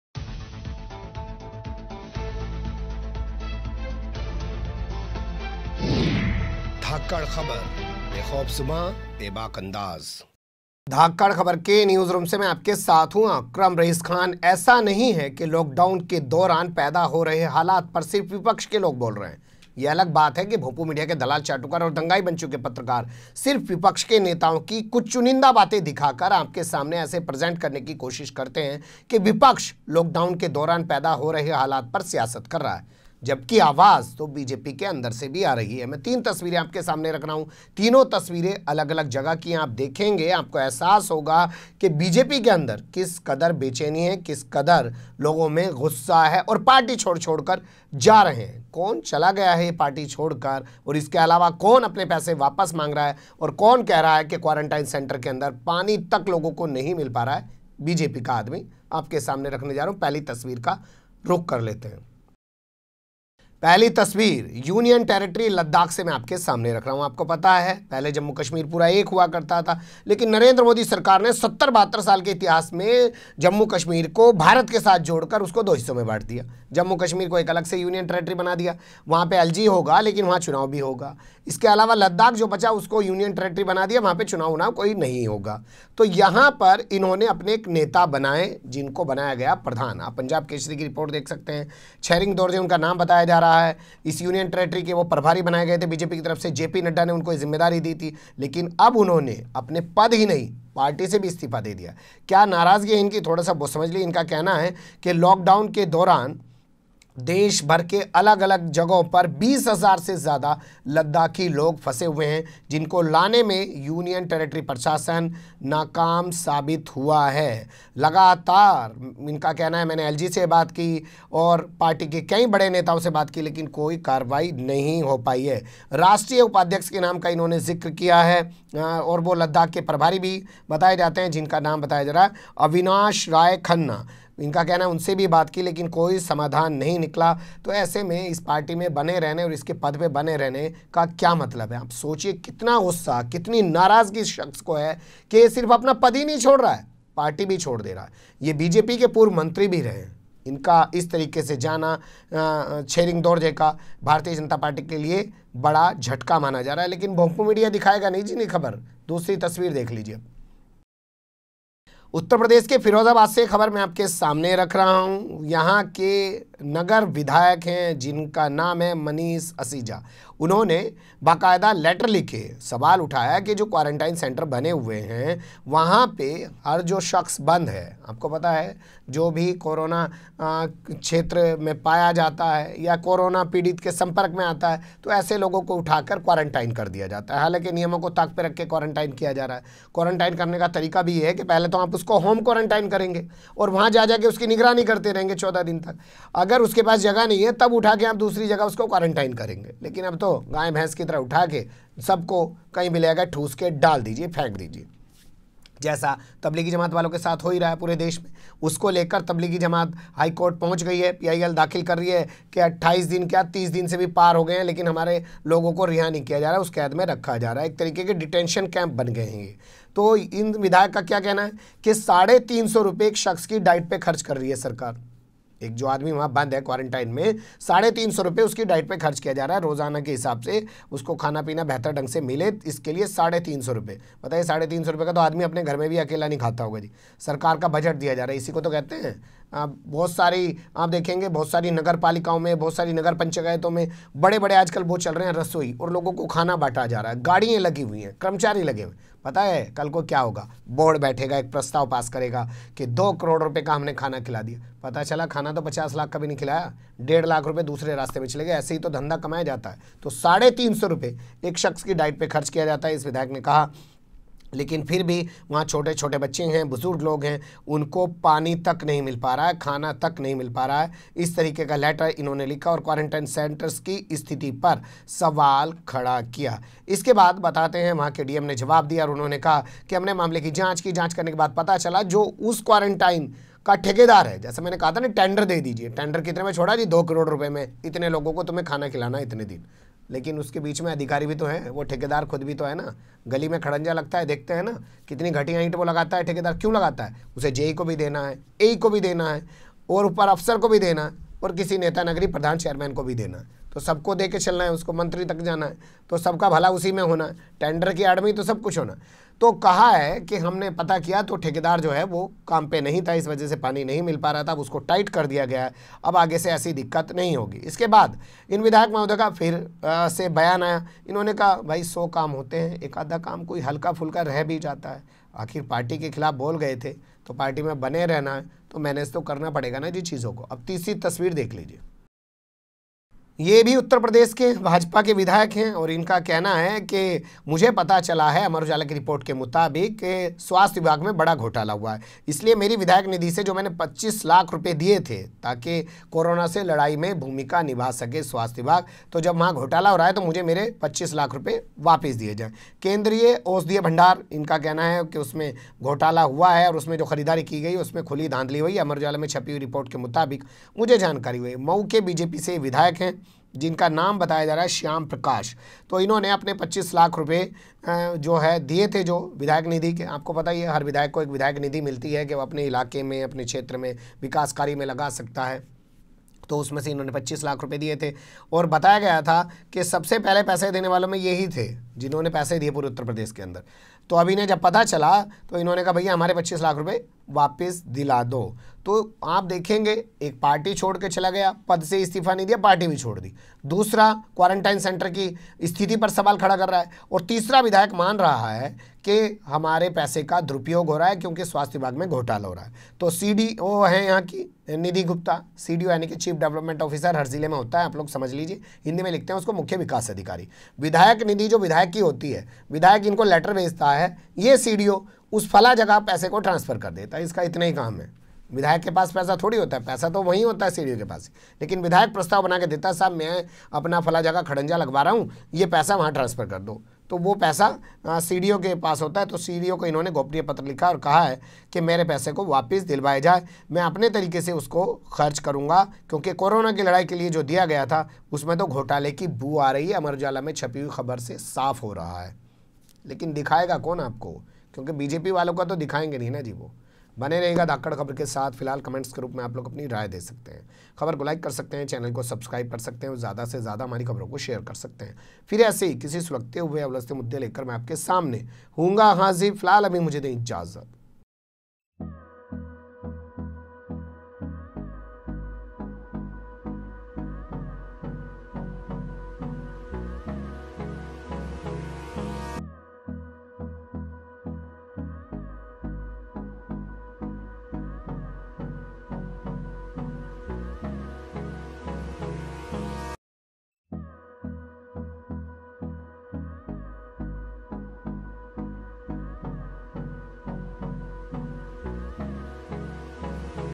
Not only opposition, BJP leaders are also questioning their government
ऐसा नहीं कि कोरोना और लॉकडाउन को लेकर सिर्फ विपक्ष के नेता ही सवाल उठा रहे हैं, बीजेपी के नेता भी गंभीर सवाल उता रहे हैं, अपने चरित्र के मुताबिक मीडिया दिखा नहीं रहा है, पर धाकड़ खबर बीजेपी के कुछ नेताओं का हाल आपके सामने रख रहा है, देखिए इस रिपोर्ट में